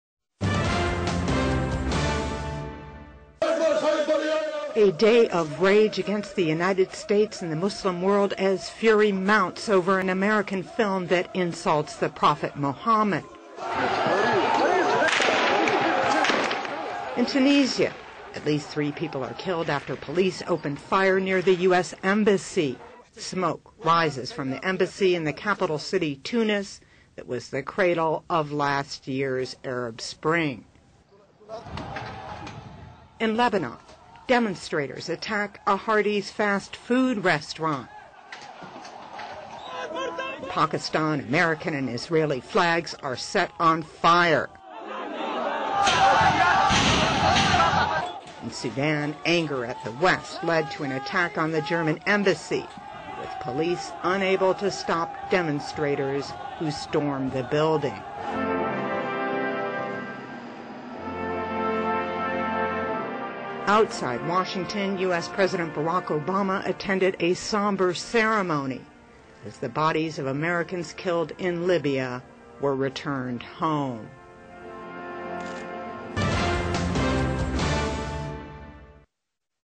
2012/9/15 反米行動がイスラム圏に拡大 ムハンマド侮辱の映像に抗議
反米行動がイスラム圏に拡大 　ムハンマド侮辱の映像に抗議（字幕・2012年9月15日）
(01:19)
米国で制作された、予言者ムハンマドを侮辱した映像に抗議するデモがイスラム圏全体に広がり、一部の国では死者も出ている。